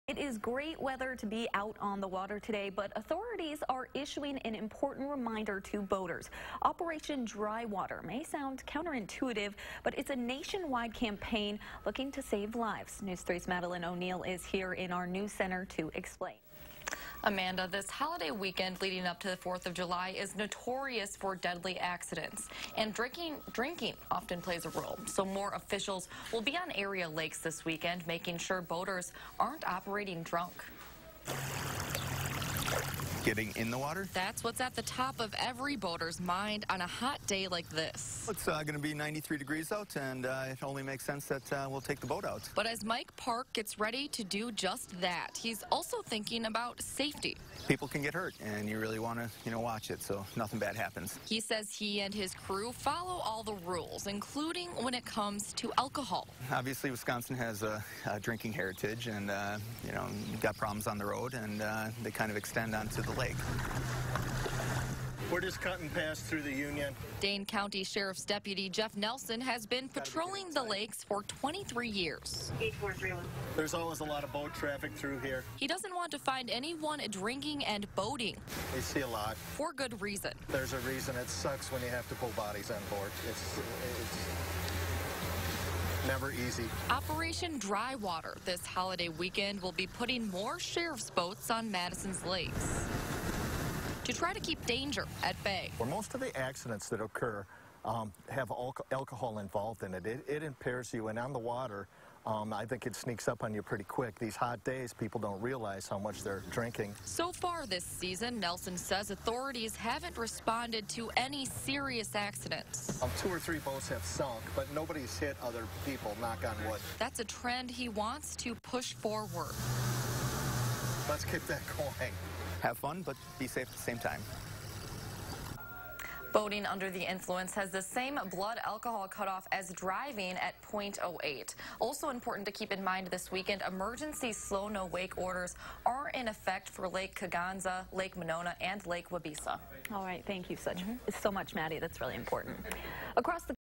Operation Dry Water aims to keep drinking and boating at bay
The Dane County Sheriff’s Office and other agencies are participating in the campaign, putting additional boats on area lakes to increase enforcement and raise awareness about the dangers of BUIs.